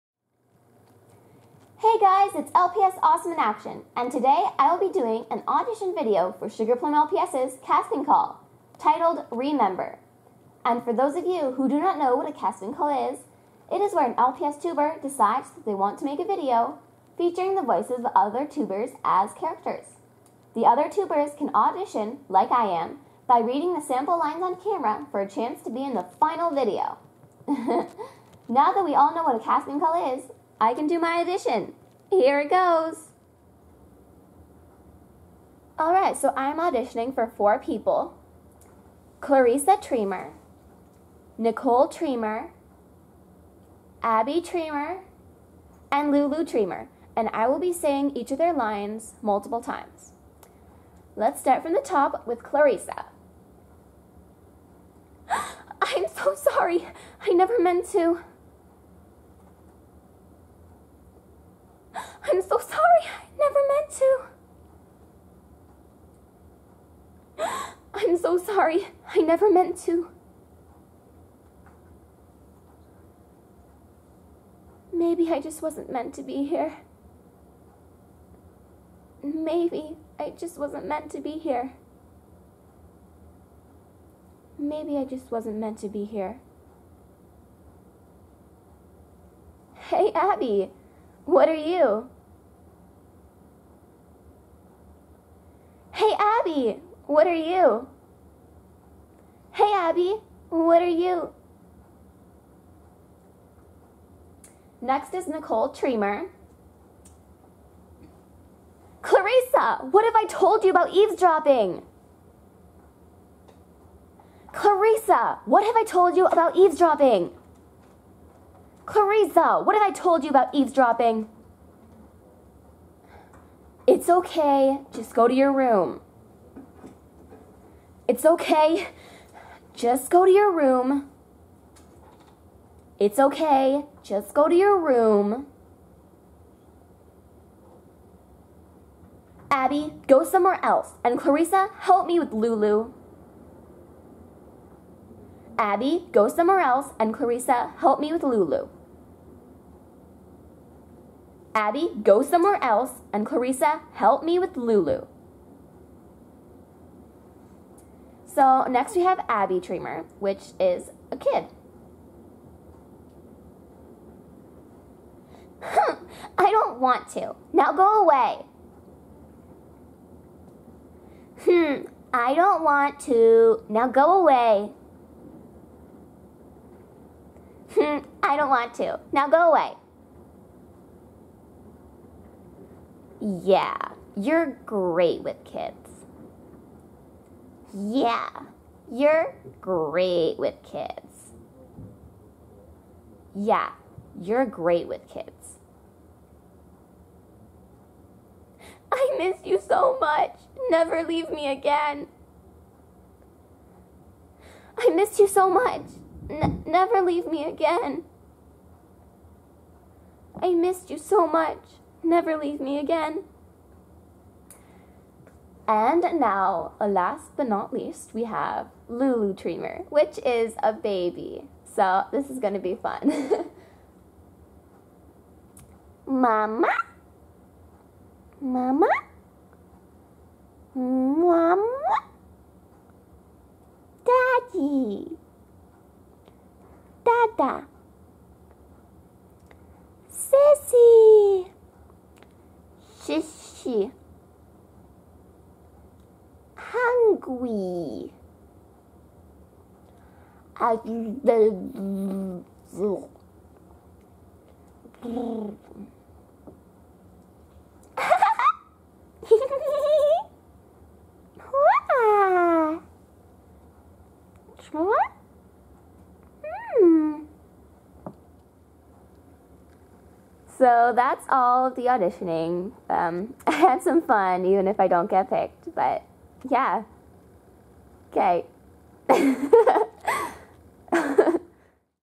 Casting Call Audition "Remember" For SugarPlumLPS
Hey guys its LPS Awesome In Action and today I will be doing an audition video for sugarplumplps's casting call, titled "Remember". And for those of you who do not know what a casting call is, it is
where an LPS Tubers decides they want to make a video featuring the voices of other tubers as characters. The other tubers can audition by reading the sample lines on camera for a chance to be in the final video!
And that is exactly what I will be doing today! Just click that play button to hear an arrangement of phrases in different crazy voices!
Enjoy! xD

And yes I didn't put my outro or intro. I was short on time  :3
...and now I realize I also forgot to put music :P

Now... I must figure out why this took me an hour to edit! O_o

(The NTM should be up soon as well)